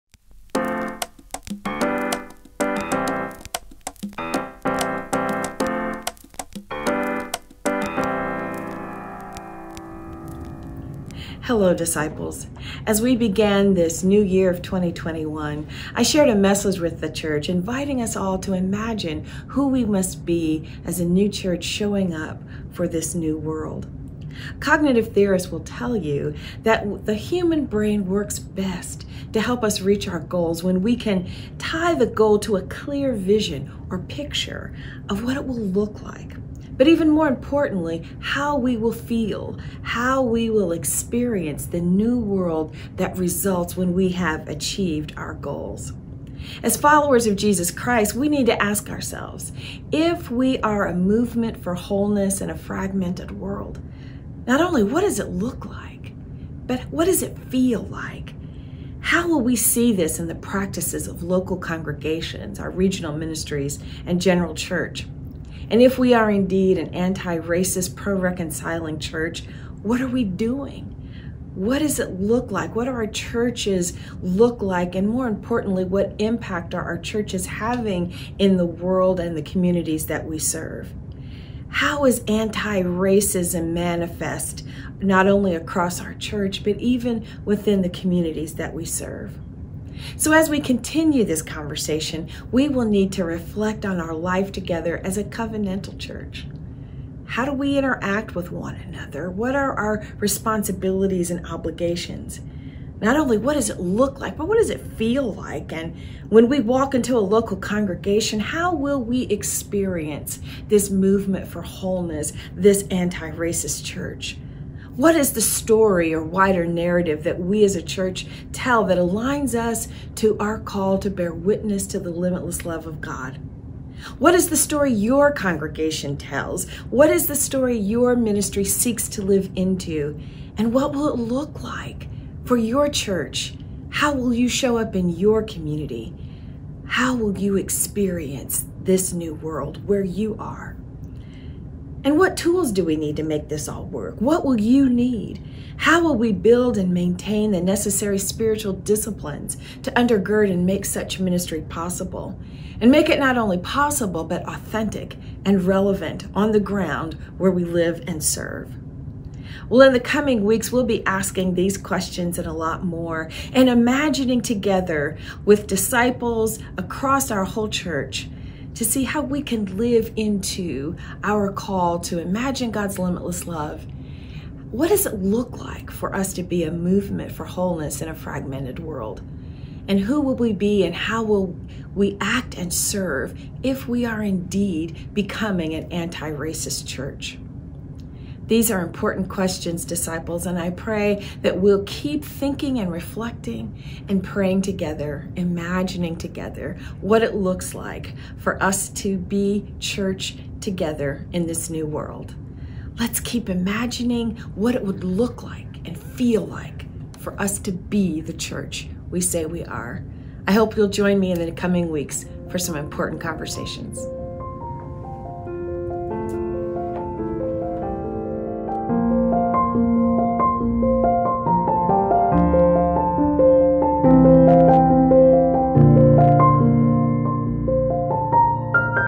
Dear Disciples, January 29, 2021
General Minister and President Terri Hord Owens invites Disciples to imagine what it would look like for us to live into our call to be a movement for wholeness and an anti-racist church.